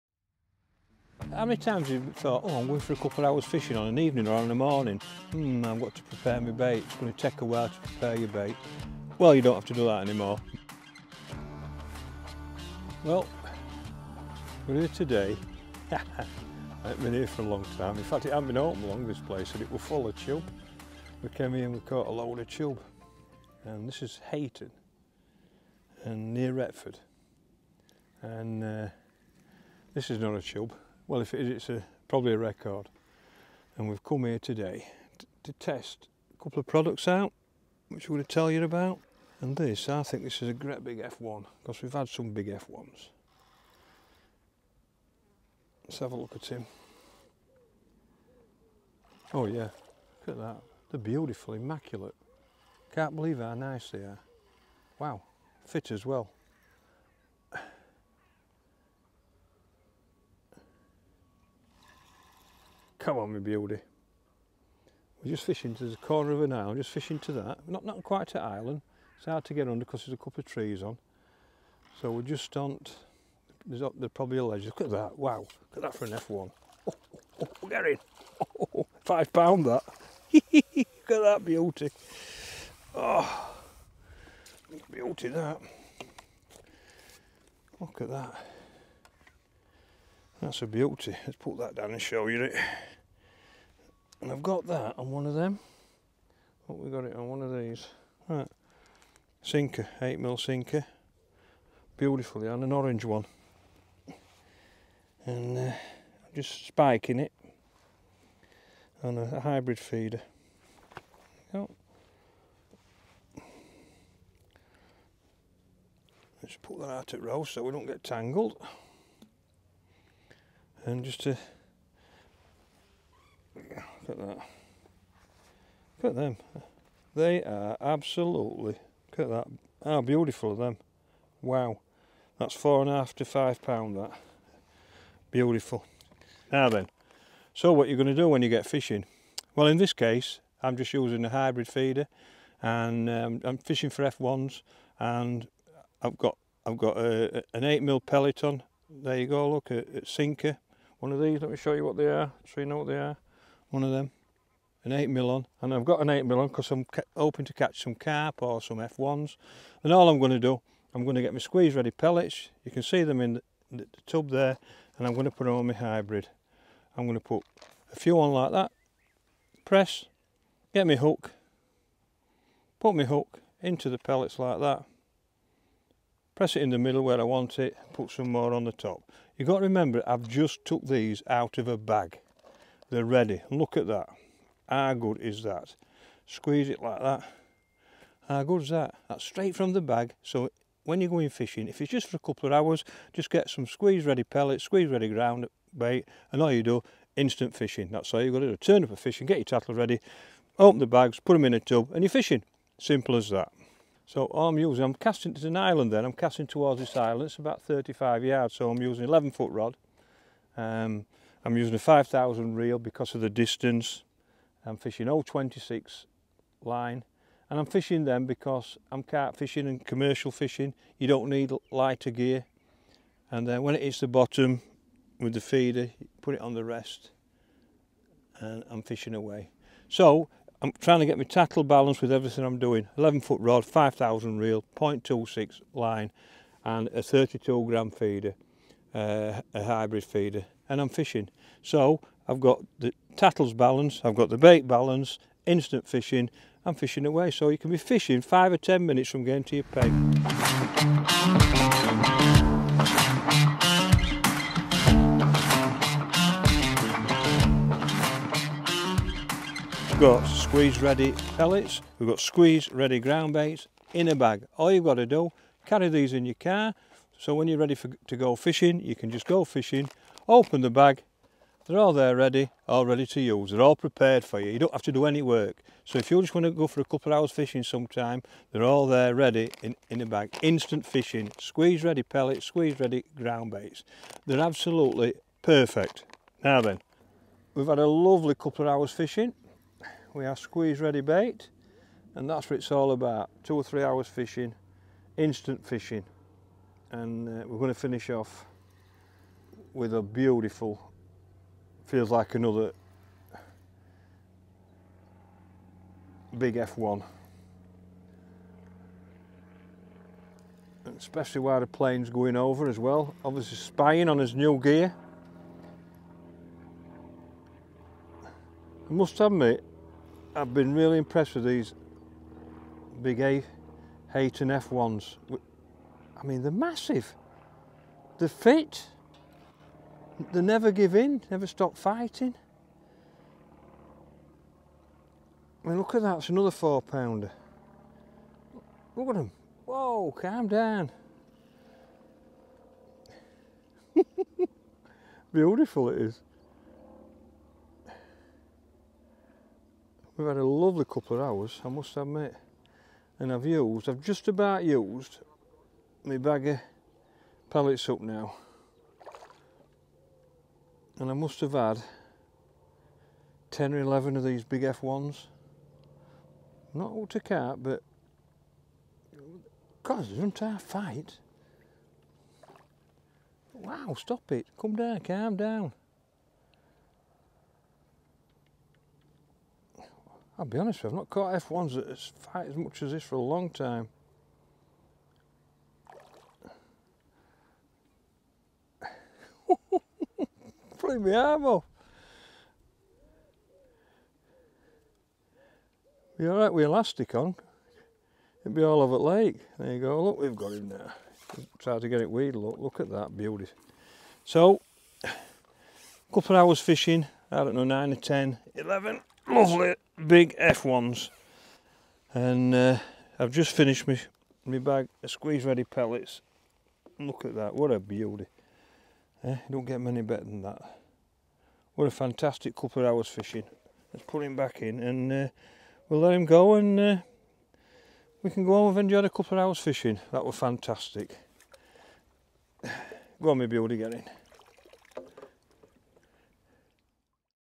Feeder fishing for big F1s with NEW BAIT | Tom Pickering
Tommy PICKERING catches some GIANT F1's while testing NEW Squeez-Ready Pellets.
Get all Tommy's top tips for instant commercial feeder fishing.
Filmed at Hayton Lakes near Retford.